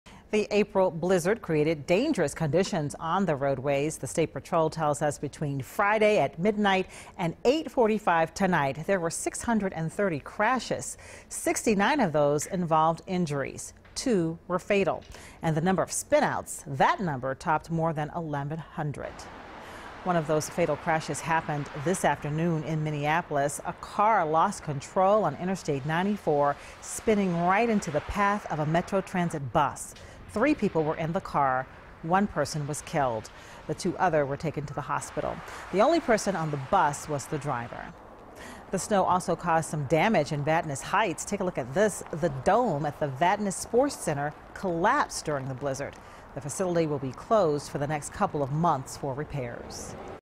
630 Crashes, 1,182 Spin-Outs Amid Record Snowstorm
Angela Davis reports on the crashes that left two people dead (0:50). WCCO 4 Weekends – April 15, 2018